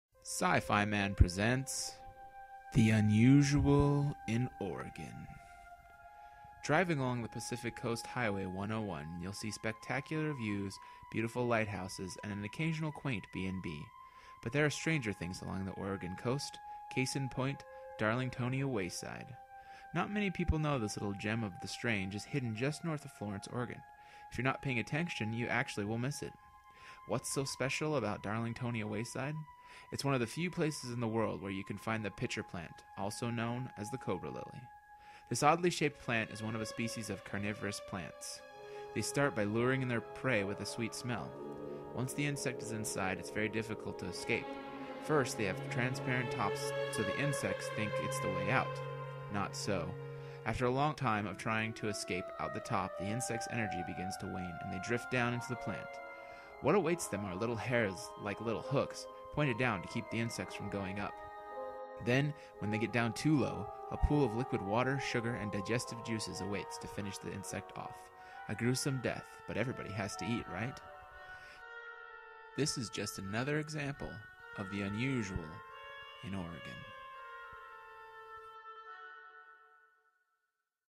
The Unusual in Oregon: Ep. 1 - Darlingtonia Wayside
In the first episode of "The Unusual in Oregon" I showcase the Darlingtonia Wayside, a quaint little spot along the Oregon coast.  If you've been there, let me know what you thought.  If not, where have you been that's "Unusual in Oregon"?